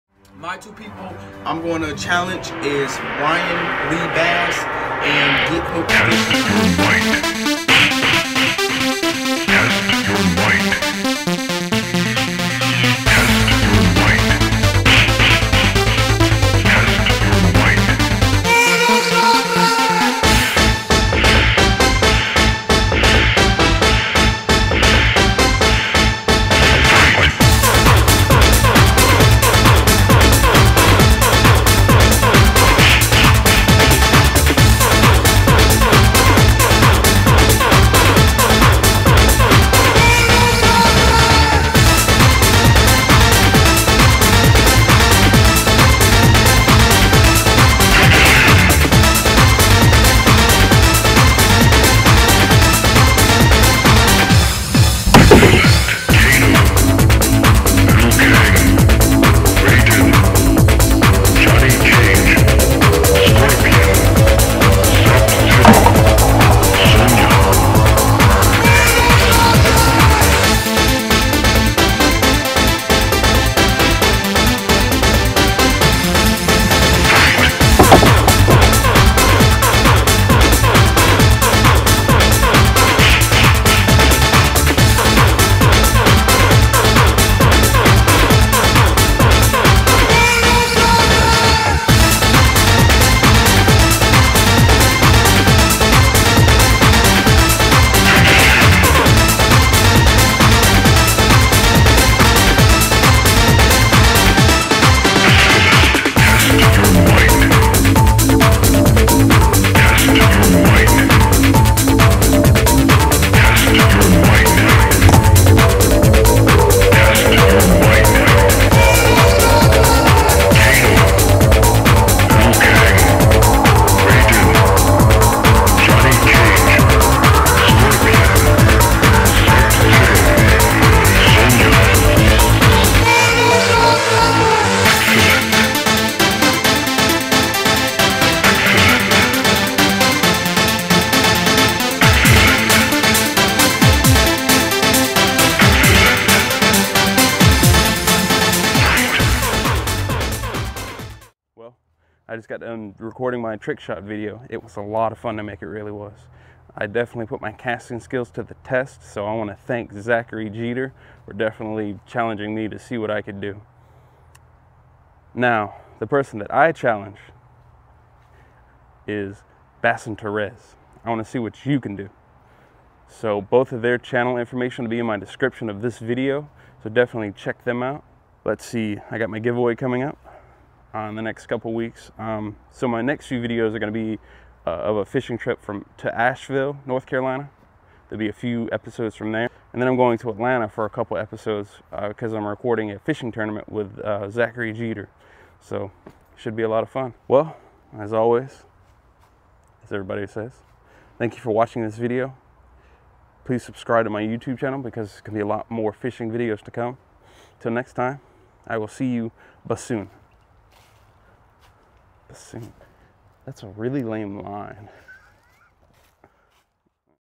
Fishing Trick Shot Challenge
In this video I undertake the fishing trick shot challenge. Enjoy!